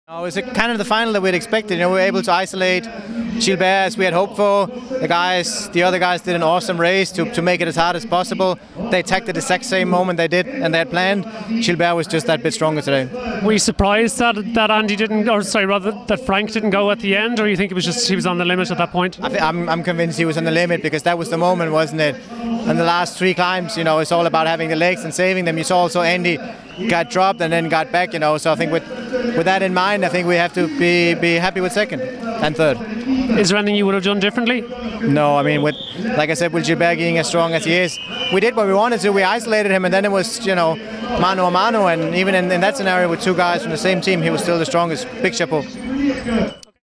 Brian Nygaard states that Schlecks gave everything in Liege-Bastogne-Liege
Leopard Trek general manager explains that he believes Gilbert was simply too strong in Liege-Bastogne-Liege